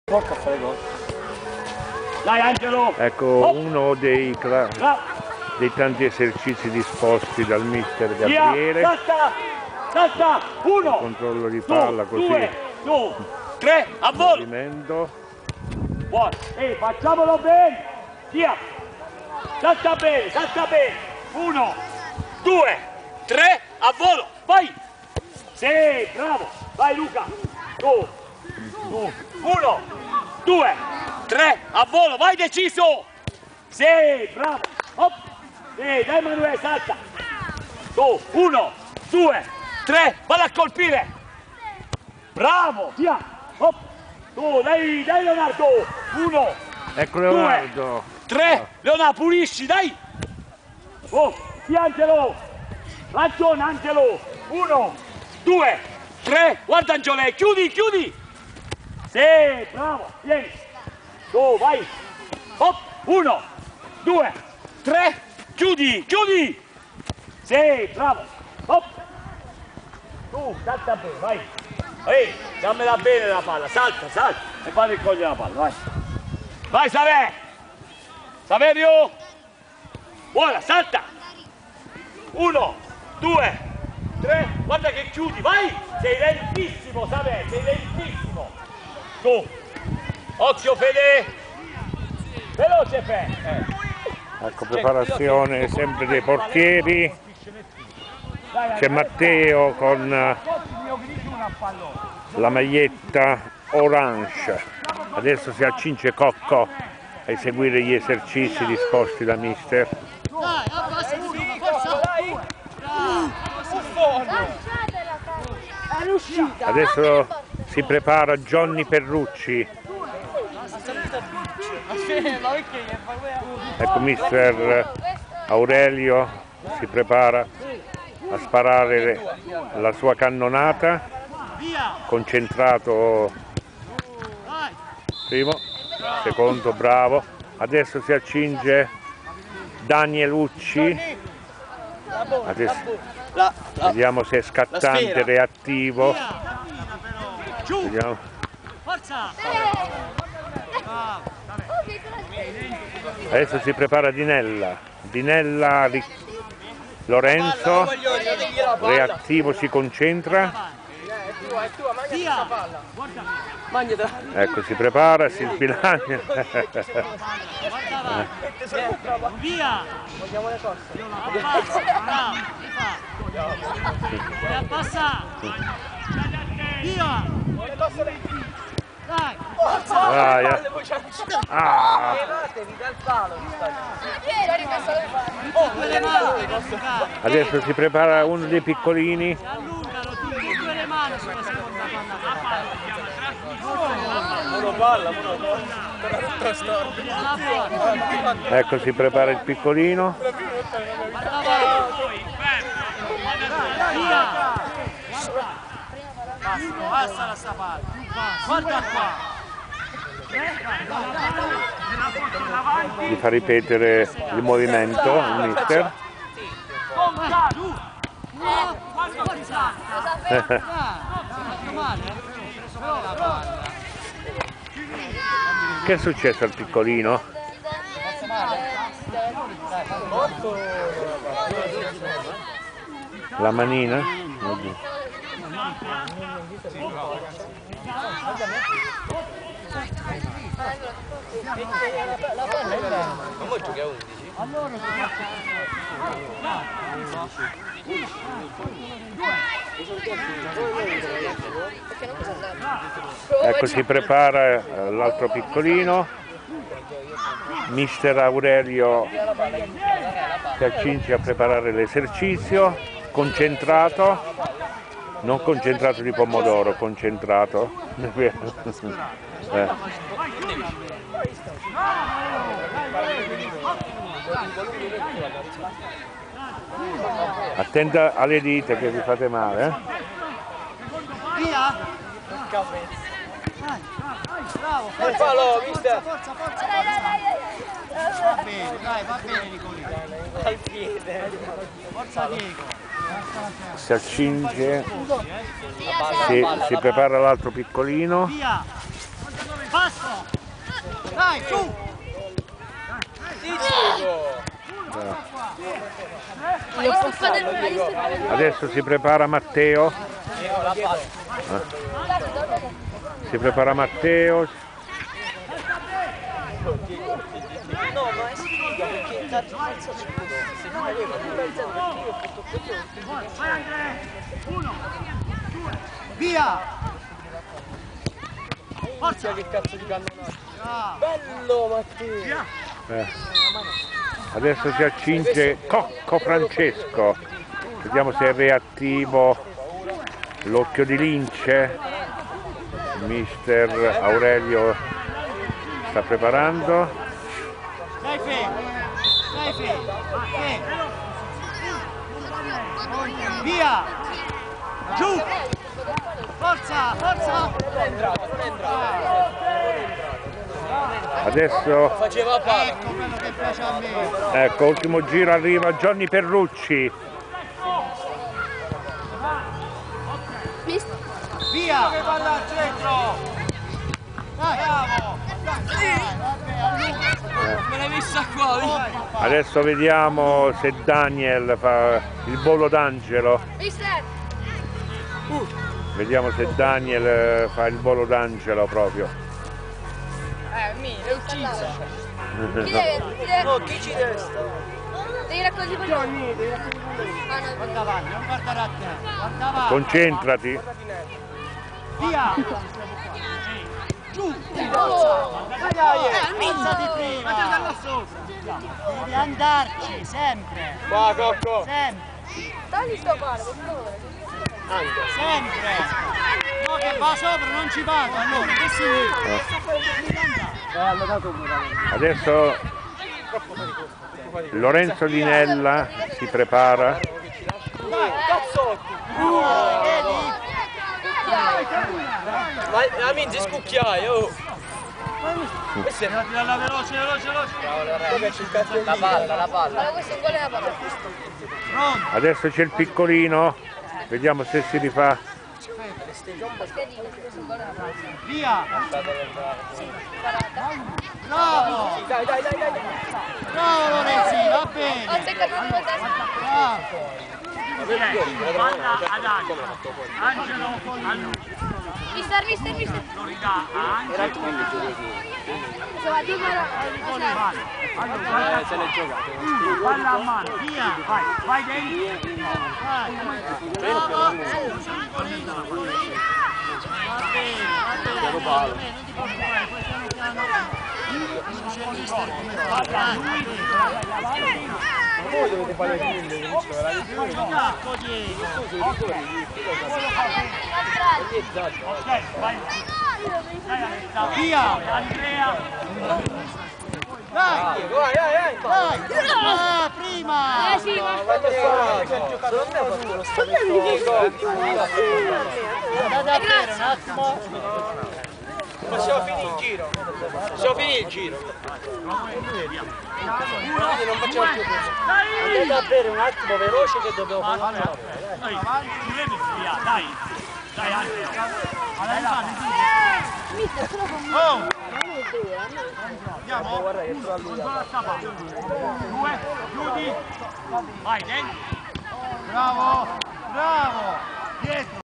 VIRTUS CASTEL FRENTANO ASD -Stag. 2017 - 2018 ALLENAMENTO PORTIERI
Oggi , Venerdì  al " Nuovo " di Via Olimpia allenamenti per i nostri baby atleti delle varie Categorie della  Scuola Calcio con i rispettivi Mister . Si sono allenati con il loro Preparatore , anche i nostri Portieri .